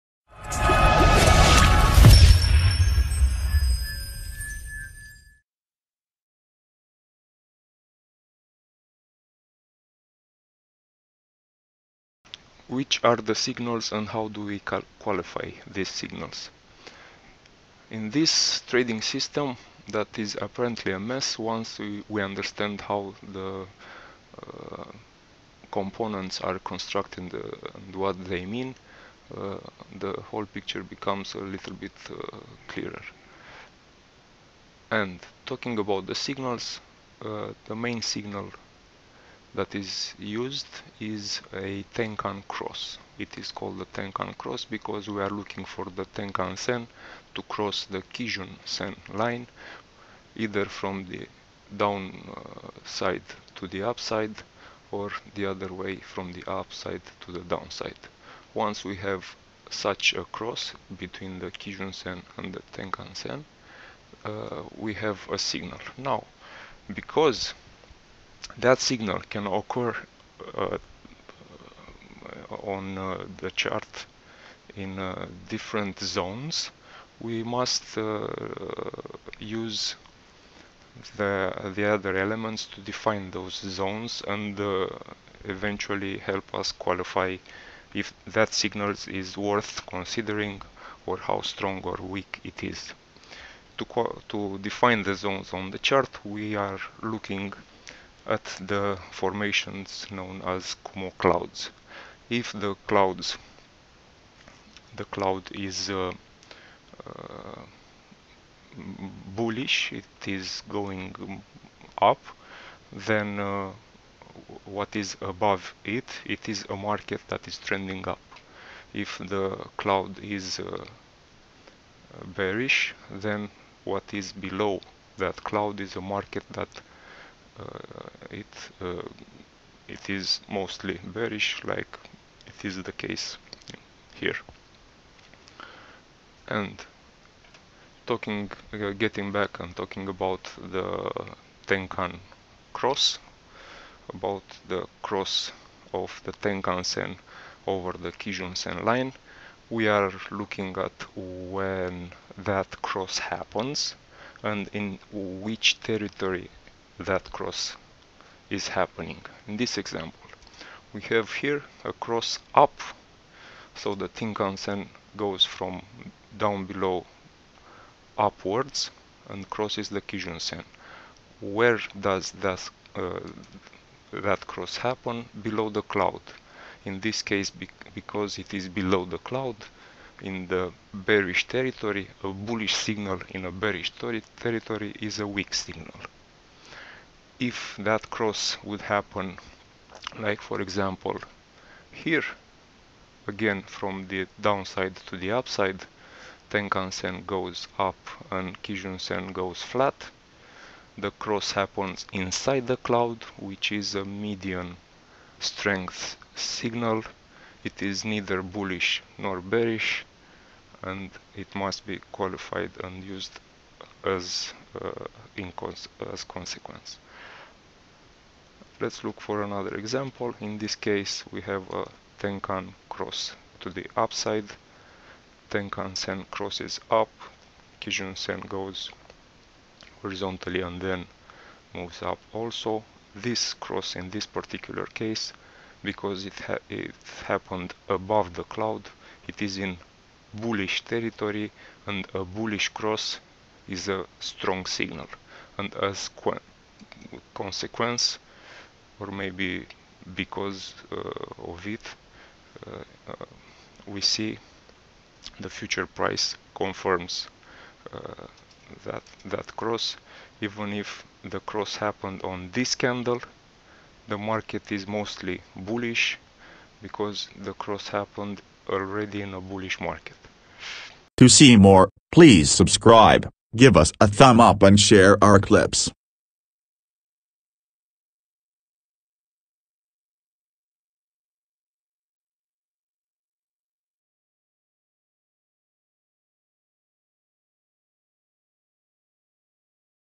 Ichimoku Cloud Trading Strategy Part 3 of 5
Part 3 out of 5 (Signals) - Ichimoku cloud, otherwise known as Ichimoku Kinko Hyo is a type of technical analysis which was started in the year 1960's. This is a very helpful indicator to determine trends and direction of the market. This strategy combines several essential theories in trading like supports and resistances, moving averages, bearish and bullish signals. It was described as one look equilibrium chart.
A lot of beginners find this concept as very complicated, however, when you can separate each element, you will find it easier for you to understand and execute. To learn more about this, please click the video, watch and learn.